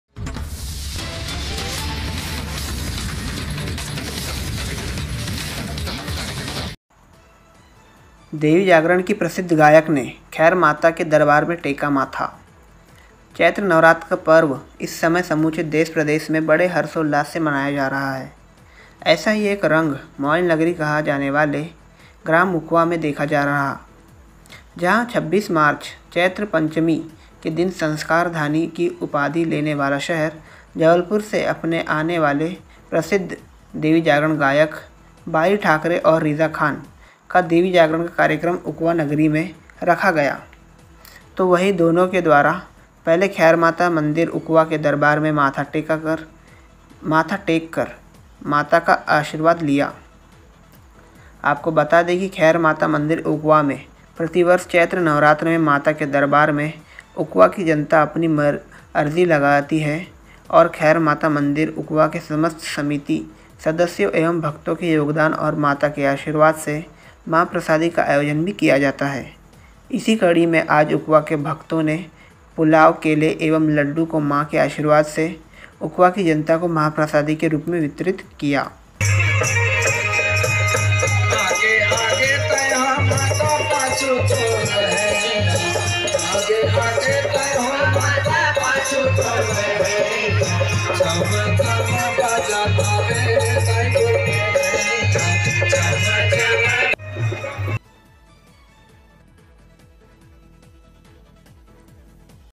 देवी जागरण की प्रसिद्ध गायक ने खैर माता के दरबार में टेका माथा, चैत्र नवरात्रि का पर्व इस समय समूचे,
*देवी जागरण की प्रसिद्ध गायिका रिजा खान, डॉली ठाकरे ने खैर माता के दरबार में टेका माथा*

*उकवा बालाघाट से अशोक कामरे के साथ रिपोर्टर निखिल राज की रिपोर्ट ।।*